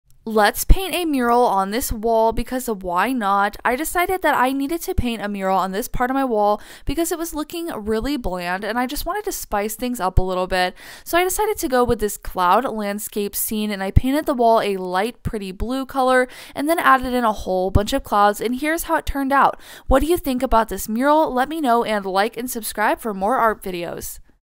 Painting a mural on my Wall 😱
This wall was looking pretty plain so I decided to give it a makeover with some paint! What do you think about this cloud mural?